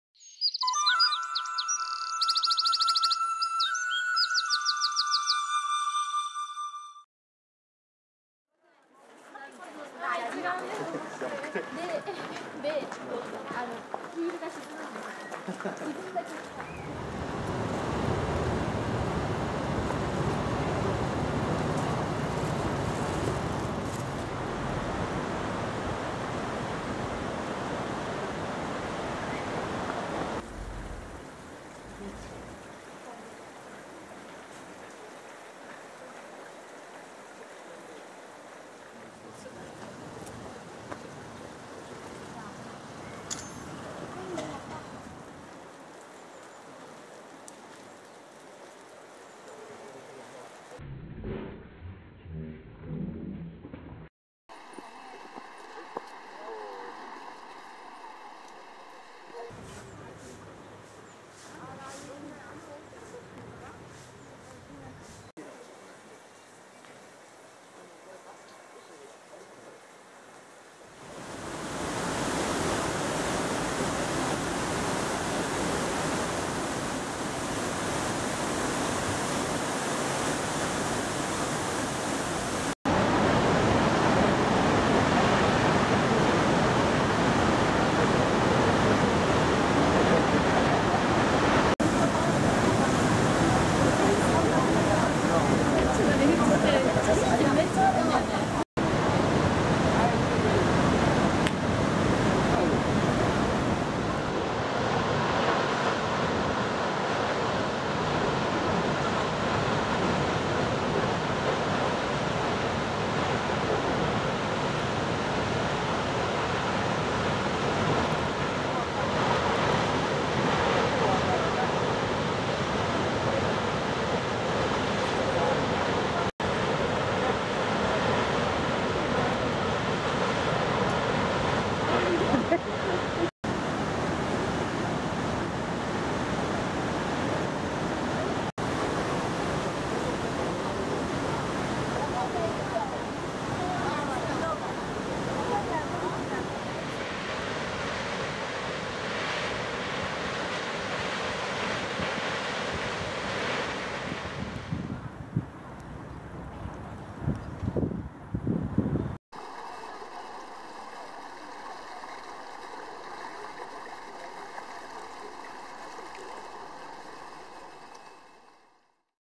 Sights & Sounds of Nikko (Autumn 2015) ᴴᴰ ● 日光 栃木
► Here's a look at a few favorite spots of mine in Nikko, Tochigi Prefecture. Located less than 2 1/2 hours away from central Tokyo, it's an amazing place to go for a hike, watching the red autumn leafs, mountains, numerous waterfalls, a mountain lakes and ponds, so many great places worth your time, if you want to see some of the great nature Japan has to offer.

Nikko is also famous for its many shrines and temples, but that'll be for another time, besides, I showed a lot of places of worship recently, so it's official "Shrine Break Time".

Easy access to Nikko by Tobu Spacia Express, Car or Bus. - Enjoy!